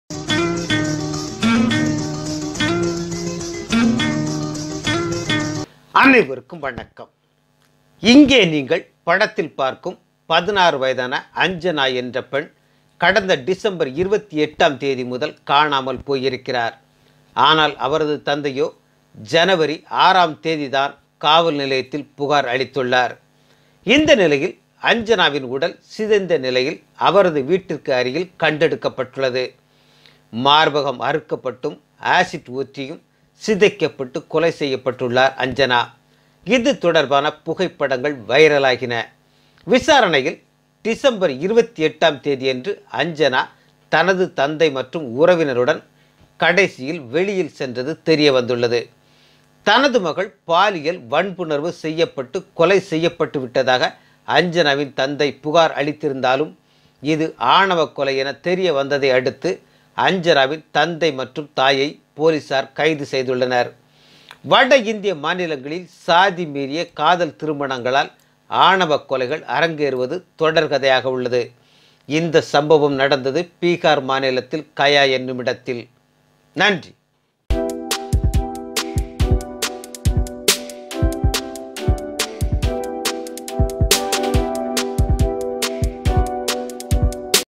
ஆணவக் கொலையின் உச்சகட்டம்! மகளின் மார்பை அறுத்து எறிந்த தந்தை!
Aanava Kolaiyin Uchakattam Magalin Marbai Aruthu Erintha Thanthai